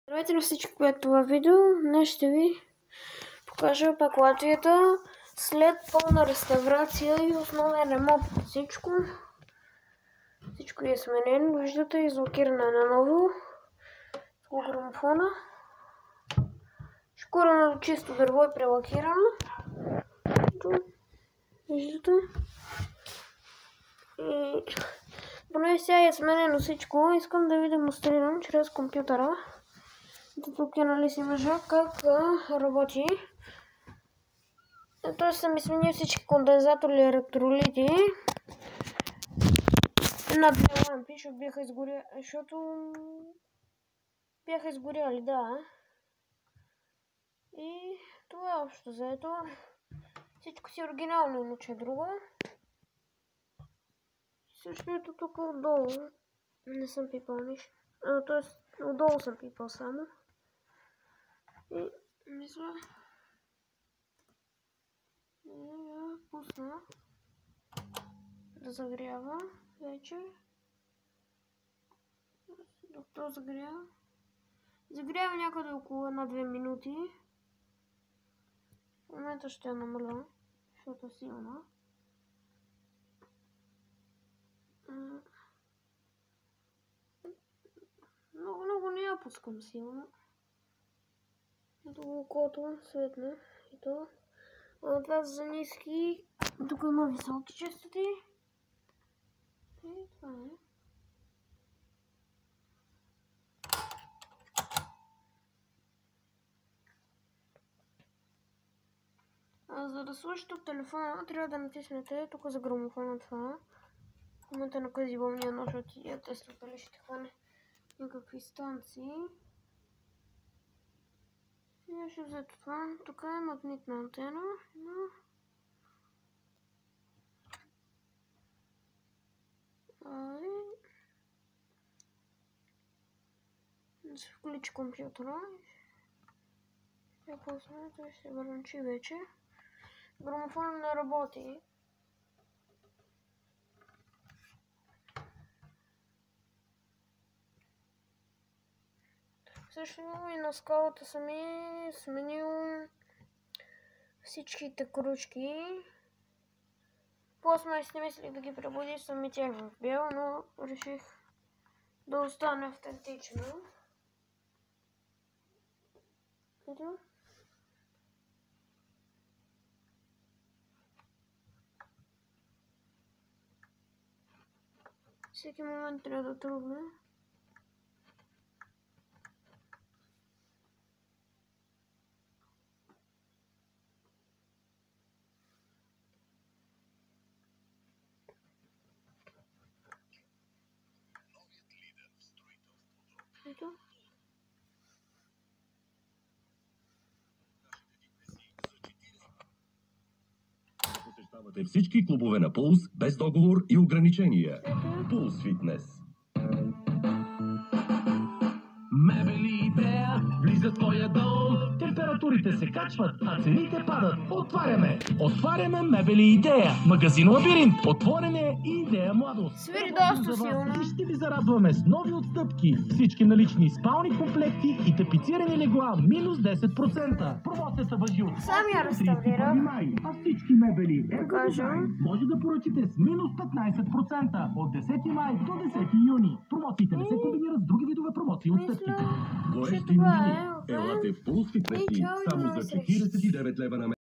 Лампов радиограмофон Латвия м след основен ремонт и реставрация
Смяна на електролитите
Смяна на кондензаторите
Смяна на 3 лампи
Шкурене до чисто дърво
Лакирана с лак за дърво
Подмяна на крушките на скалата
Стягане на кордата за търсене на станции